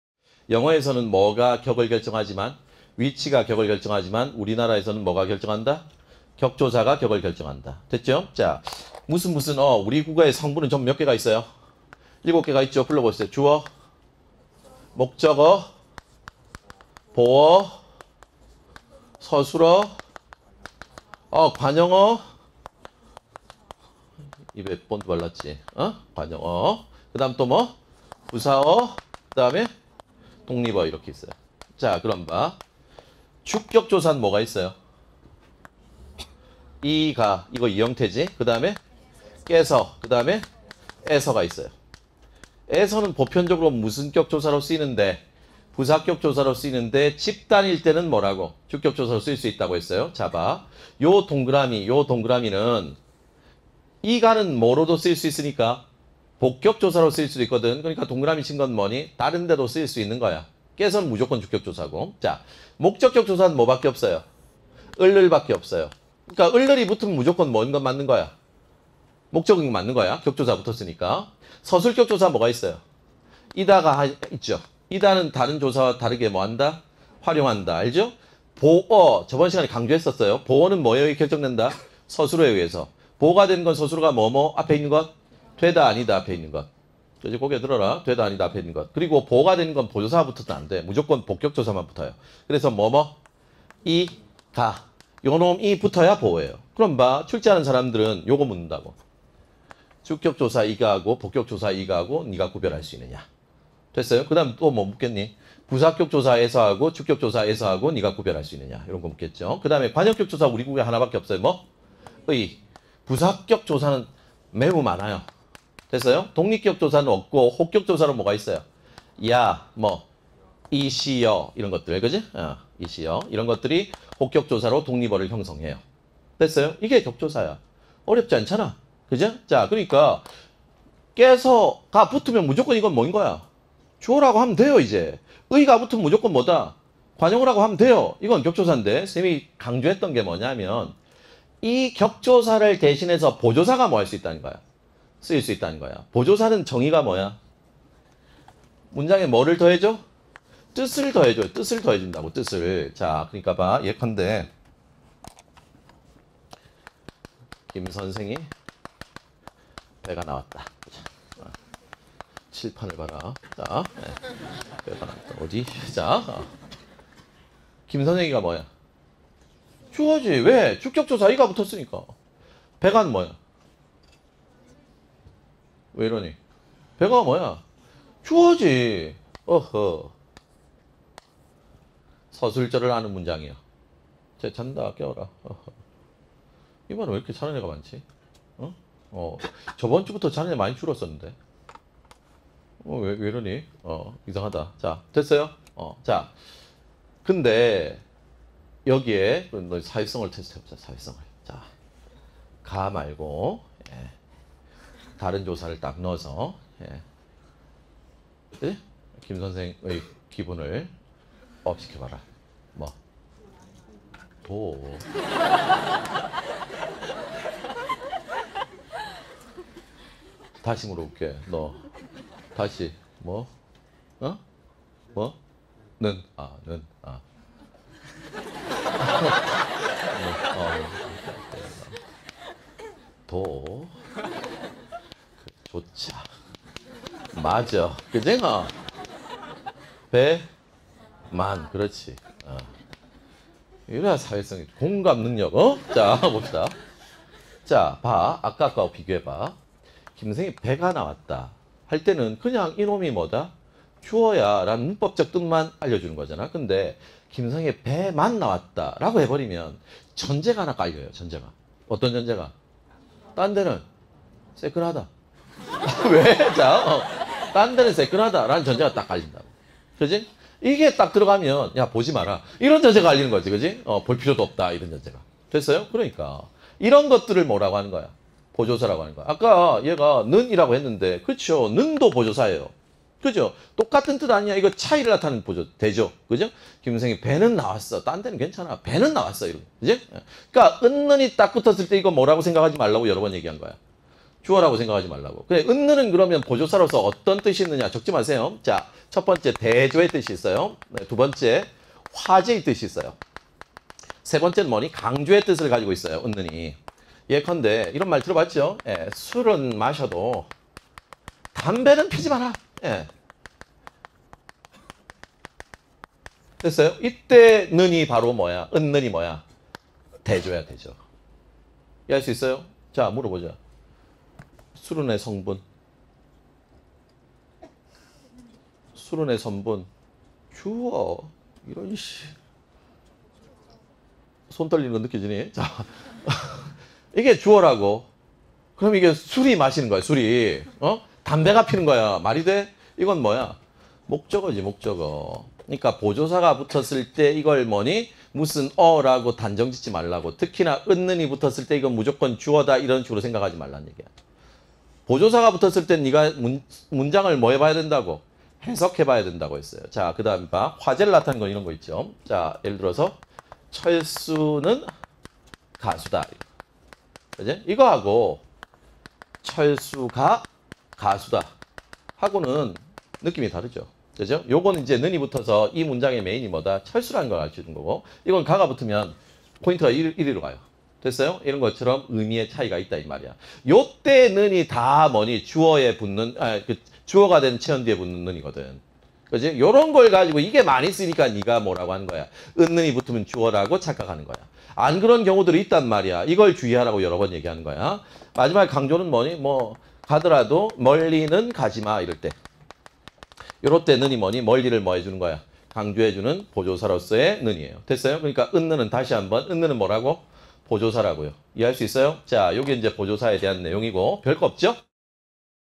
[메가스터디] 국어 김동욱 쌤 - "김선생이 배( ) 나왔다?"
조사, 김동욱 선생님의 재밌는 예시와
설명으로 쉽게 이해하세요!

김동욱 선생님에 대해 더 알고싶다면?